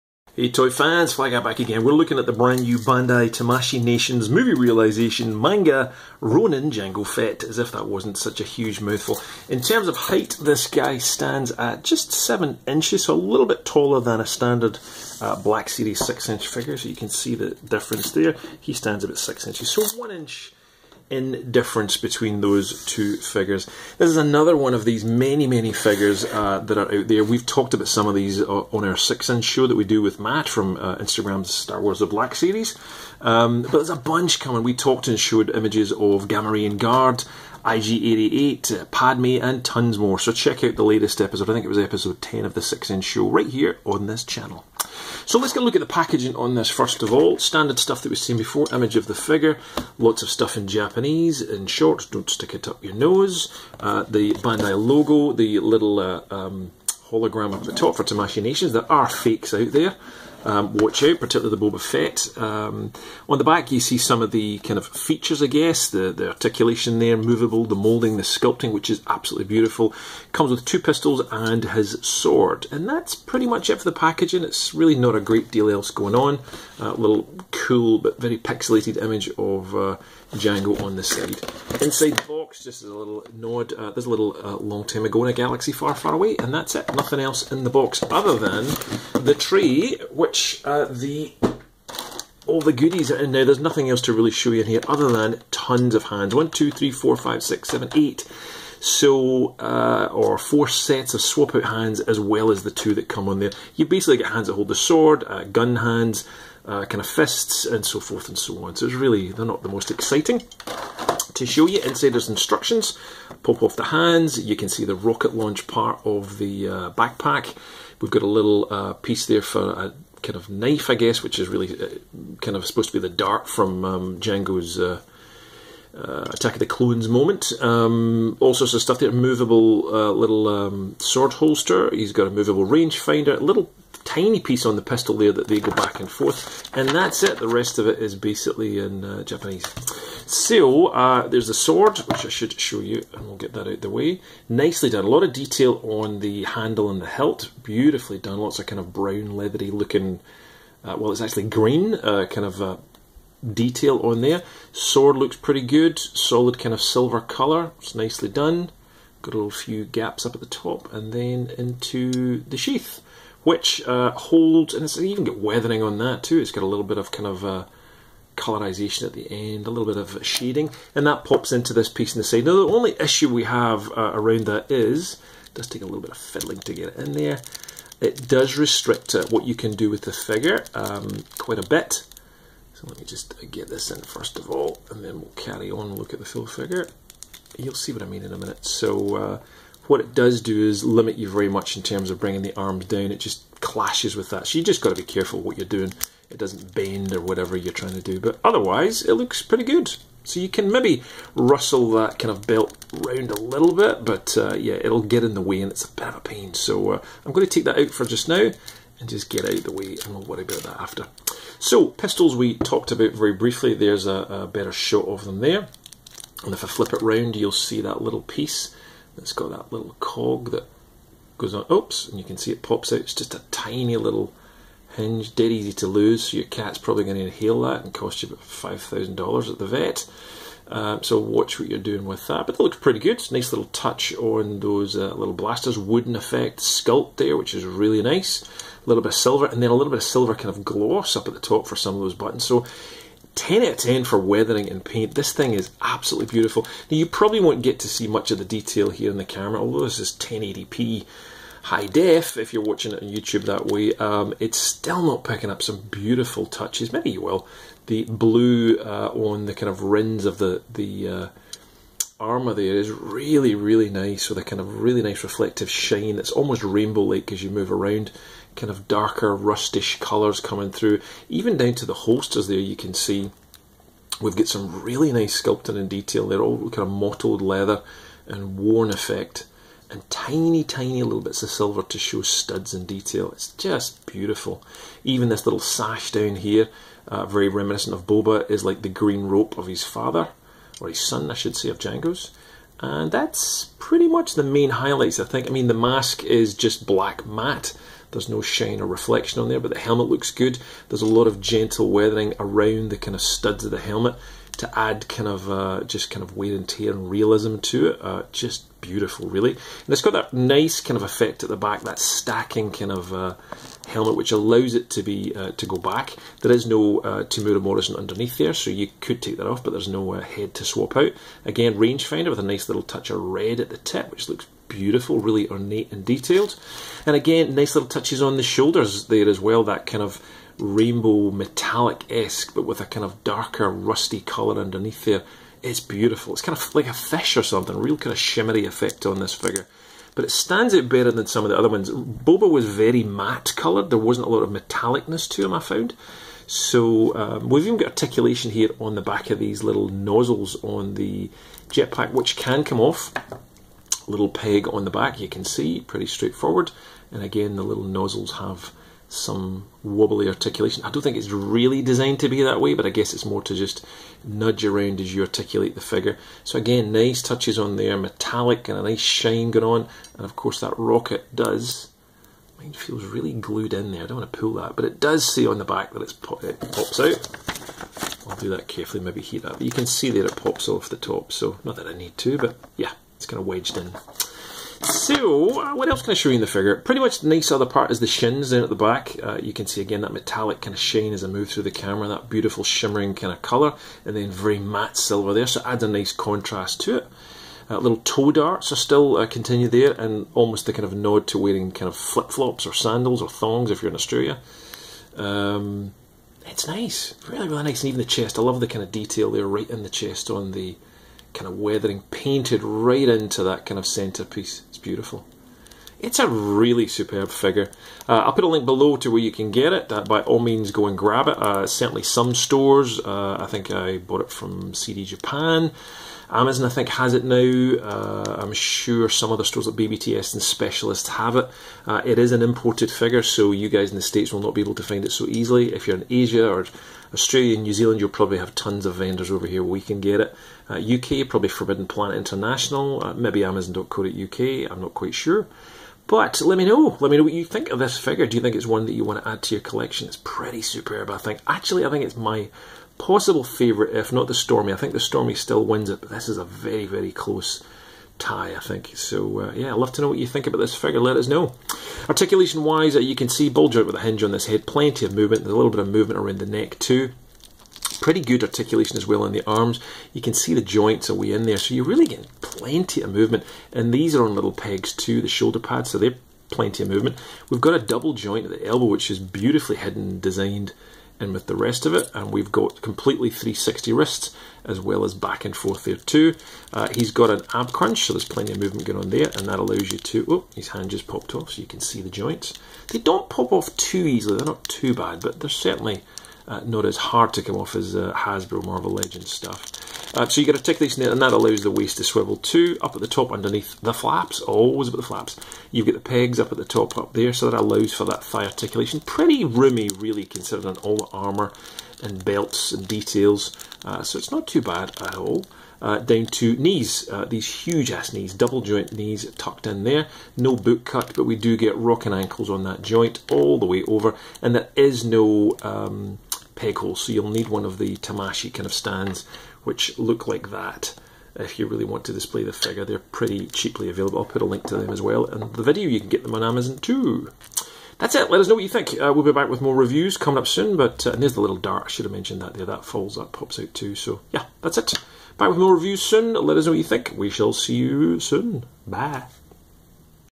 Star Wars Bandai Tamashii Nations Ronin Jango Fett Toy Action Figure Review
Includes 8 optional pairs of hands, two guns, and a kunai blade
Includes his trademark pair of guns, which can be attached to his legs
Timed for the upcoming new Star Wars film come three consecutive new releases in the meisho movie Realization series
Product bears official Bluefin Distribution logo